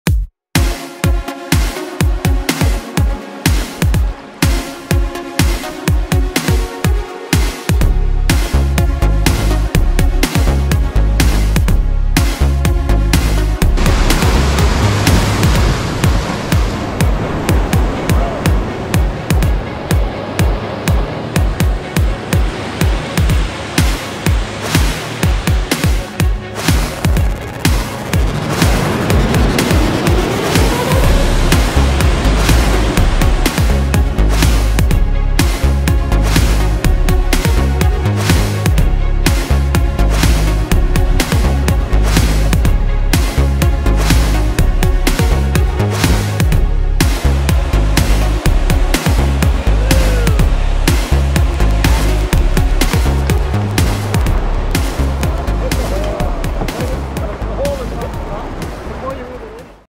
2023 Skootamatta - Short
The Skootamatta River drains the western portion of Bon Echo Park through to the Moira River to the Bay of Quinte.  The east side of Bon Echo Park drains to the Mississippi to the Ottawa River.  This is a day run downstream of Flinton to and upstream of highway 7.  Enjoy the ride!